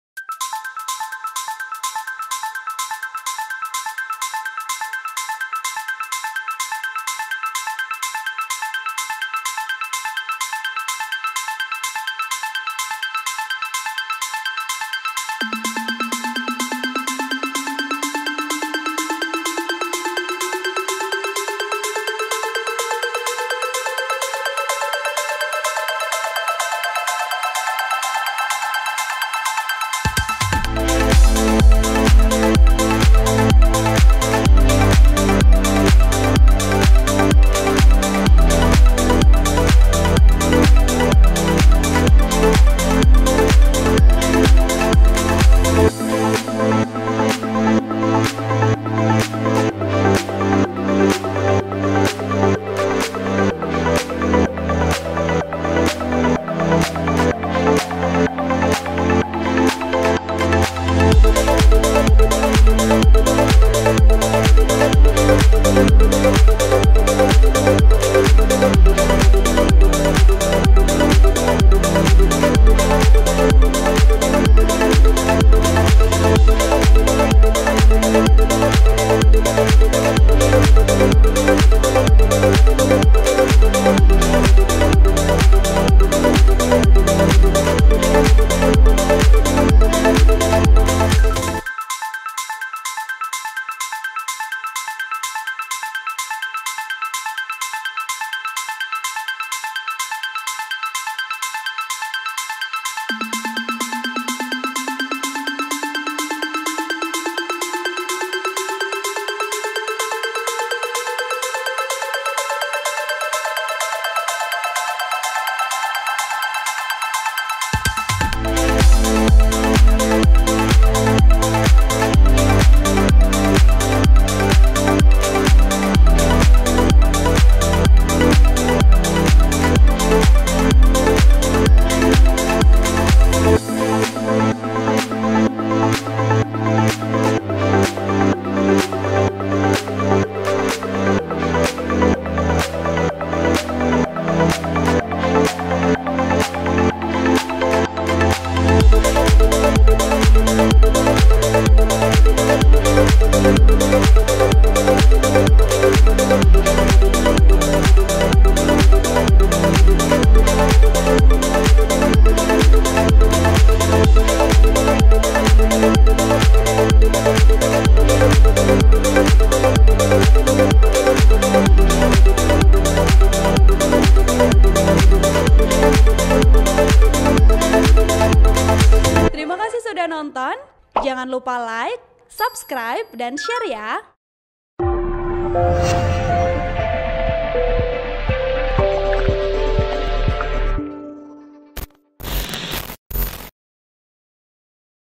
Hermus Indou, Pernah Jadi Anak Stunting Hingga Jadi Bupati Manokwari
TRIBUN-VIDEO.COM  - "Bapak yang kamu lihat sekarang ini dulunya juga stunting. Rumah gizi di Minyambouw itu yang selamatkan dia."
Begitu yang diceritakan Febelina Indou, istri Bupati Manokwari, Hermus Indou di hadapan para ibu yang tengah menggendong anak mereka yang menderita
stunting di Posyandu Bougenvil, Sowi III, Manokwari, Papua Barat, Selasa (18/10/2022) siang.

Artikel ini telah tayang di Tribunpapuabarat.com dengan judul Kisah Hermus Indou, Pernah Jadi Anak Stunting Hingga Jadi Bupati Manokwari , https:/
Penulis: Kresensia Kurniawati Mala Pasa | Editor: Tarsisius M